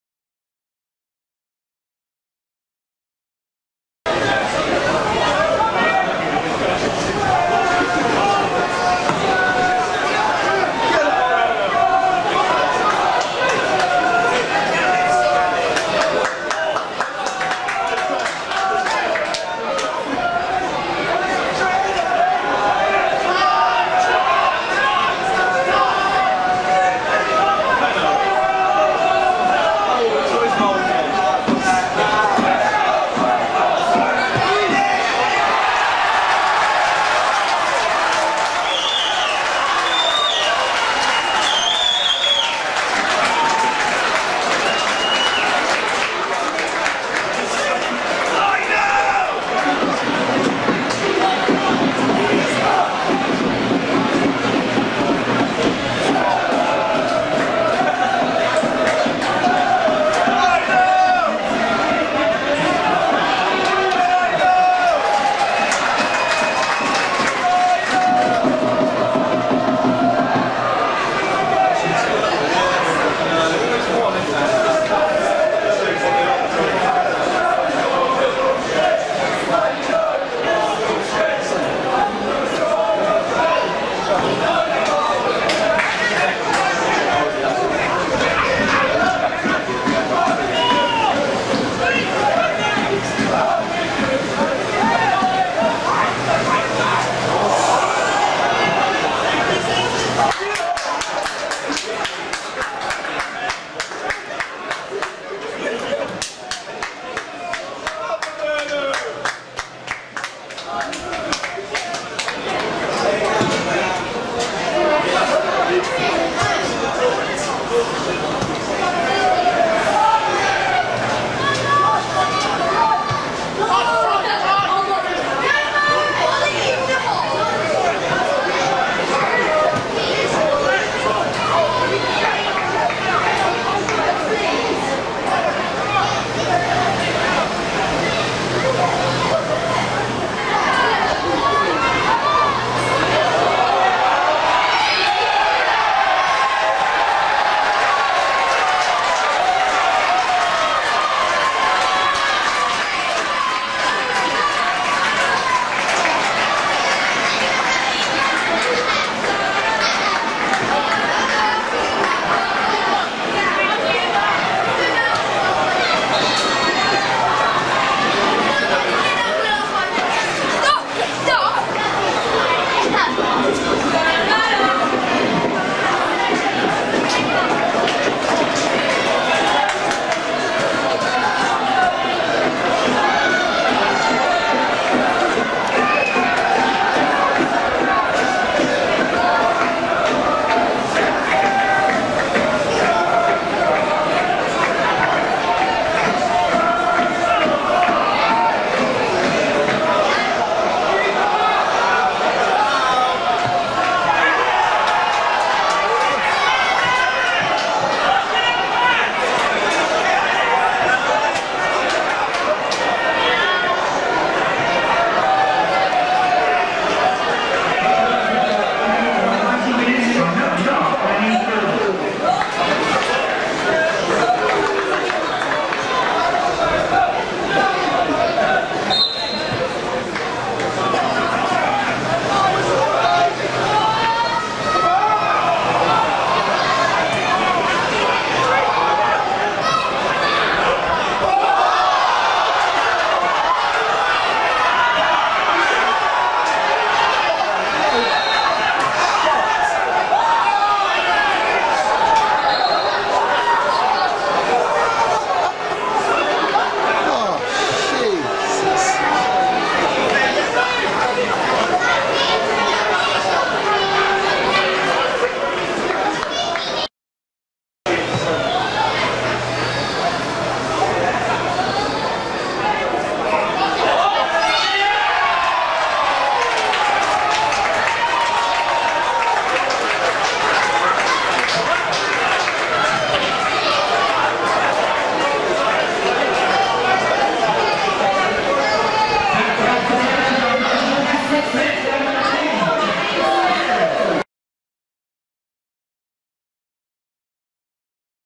Weymouth 3 v 3 Burnham - 28th March 2015
LATE drama at the Bob Lucas Stadium saw Weymouth remarkably have to snatch a point at the death against rock-bottom Burnham.